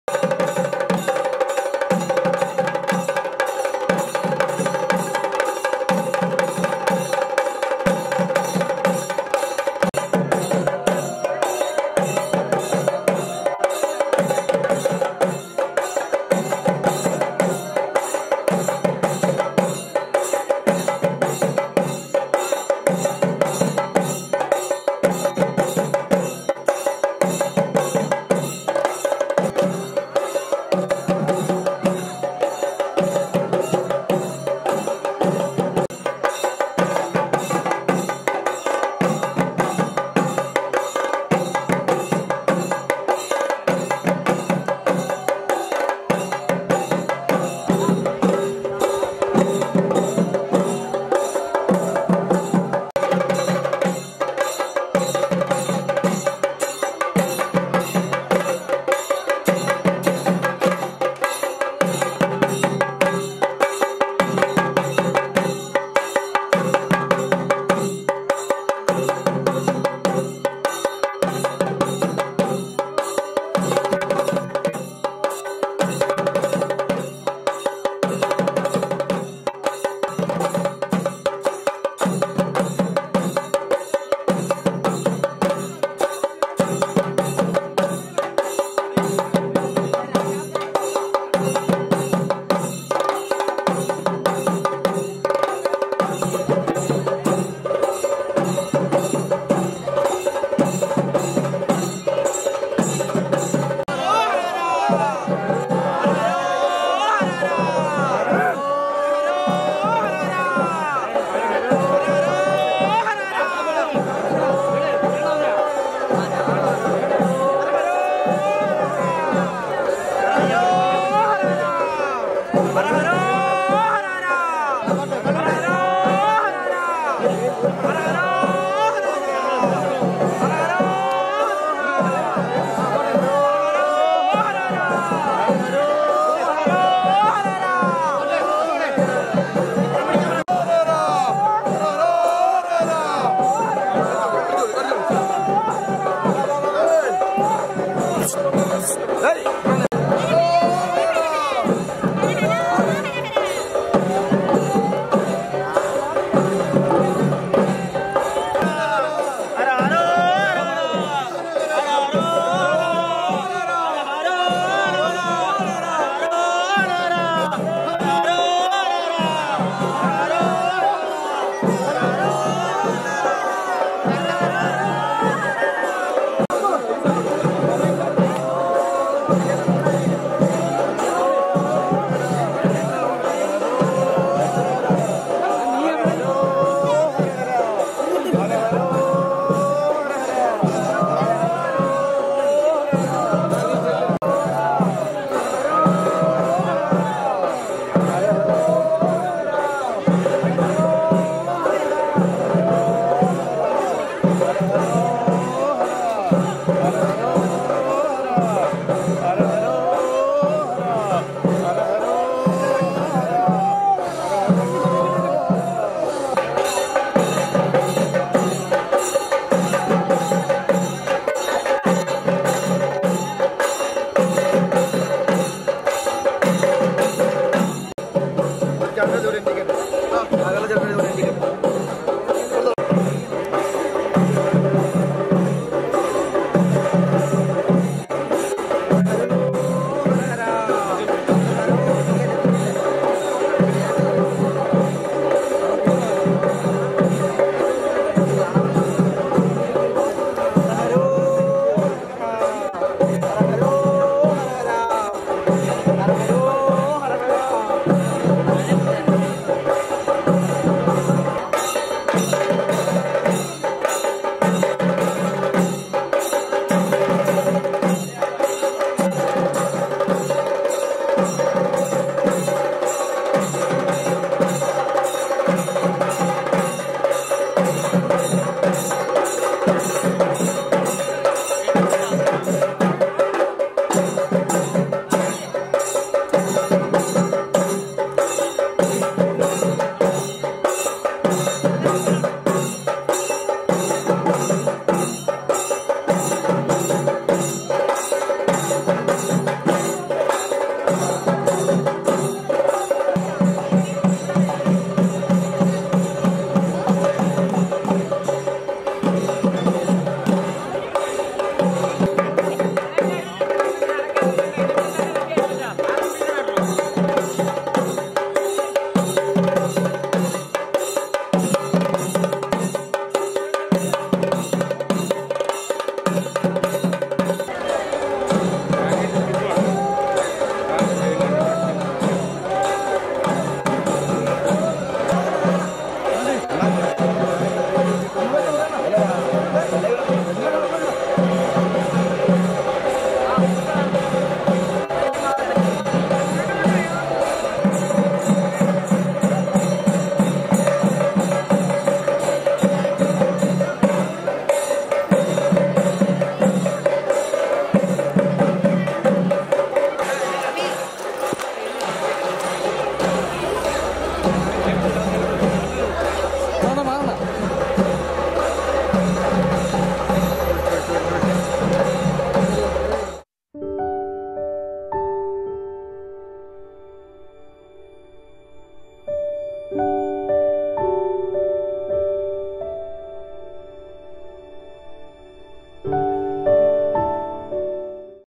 തൈപ്പൂയത്തോടനുബന്ധിച്ച് കൊല്ലത്തു നടന്ന കാവടി ഘോഷയാത്രയിൽ നിന്നും
കാമറ: ജ്യോതിരാജ്.എൻ.എസ്